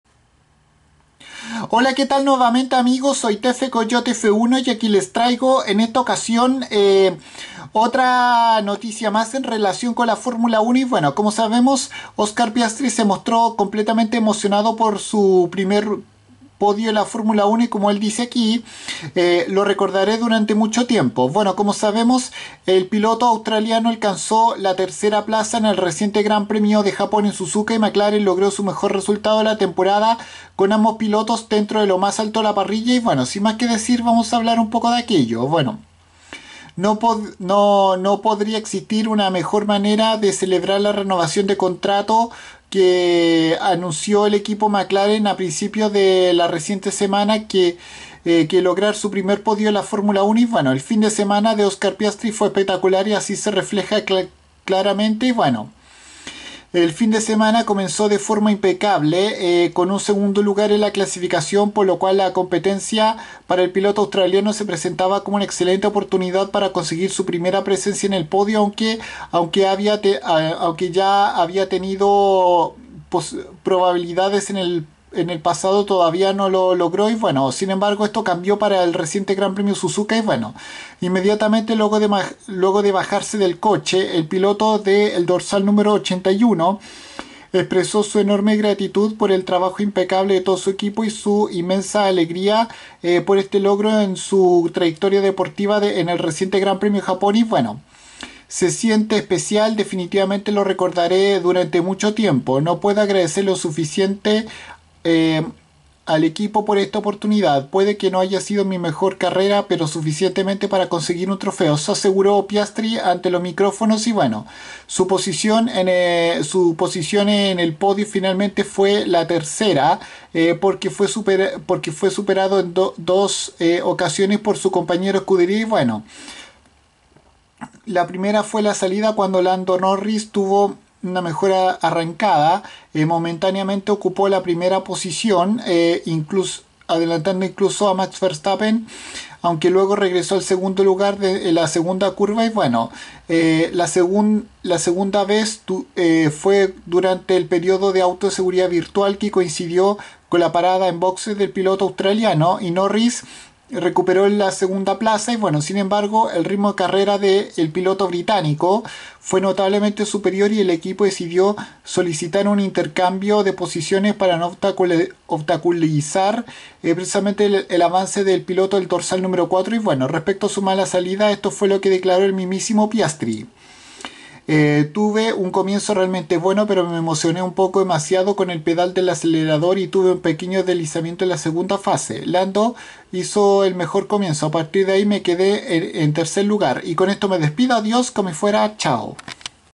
Oscar Piastri consiguió su 1er podio en la F1 en Suzuka
El piloto australiano logró por fin y dentro de su temporada debut lograr el podio en el reciente Gran Premio de Japón en Suzuka siendo este detrás de su compañero de escudería el inglés Lando Norris y del ganador el holandés Max Verstappen